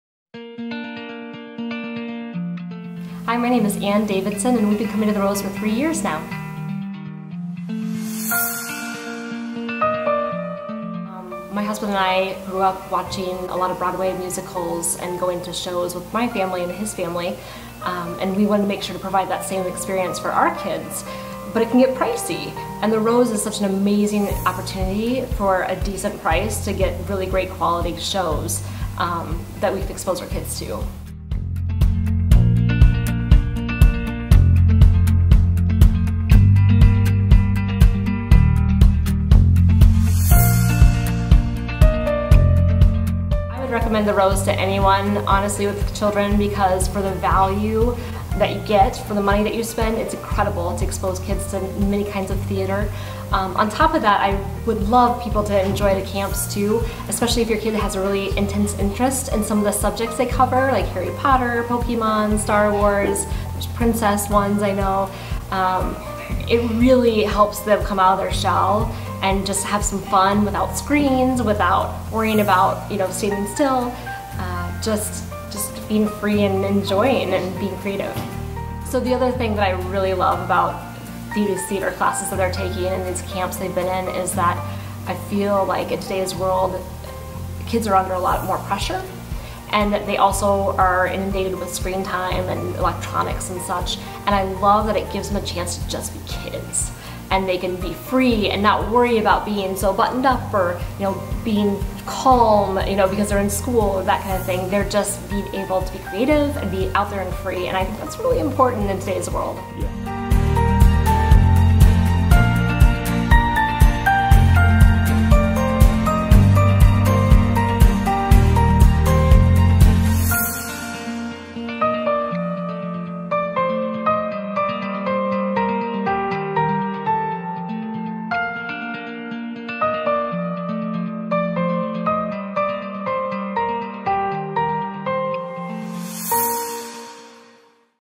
What Do You Love About Bringing Your Family To The Rose?
One of our members, Anne, was kind enough to sit down with us and tell us what she loves about bringing her family to The Rose!